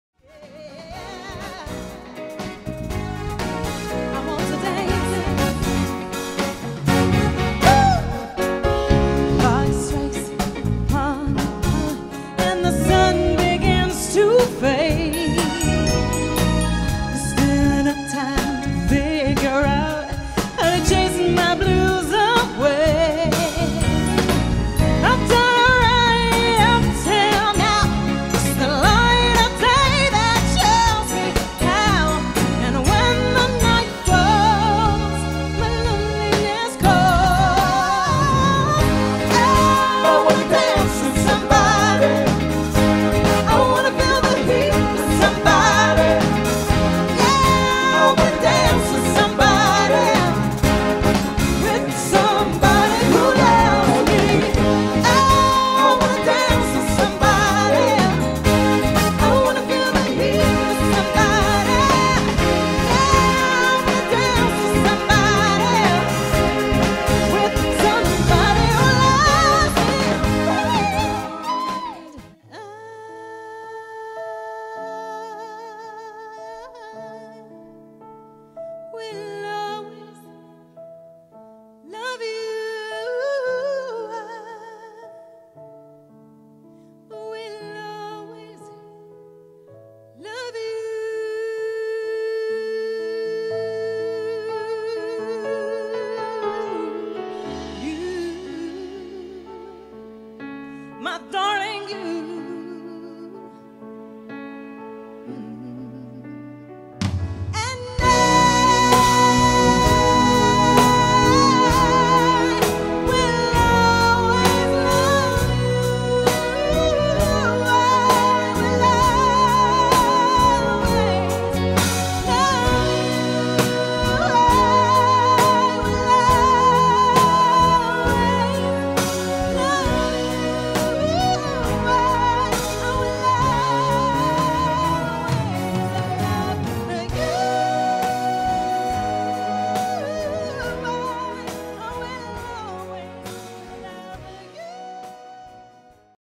I'm Every Whitney SR
Kieshia Venn showreel recorded at Keeping It Live showcase The Austin Sports And Social Club, Longbridge, Birmingham November 2018. This video is for demo purposes only. Recorded by Lincolnshire music producer Steve Allen and edited at the ark recording studio Lincolnshire.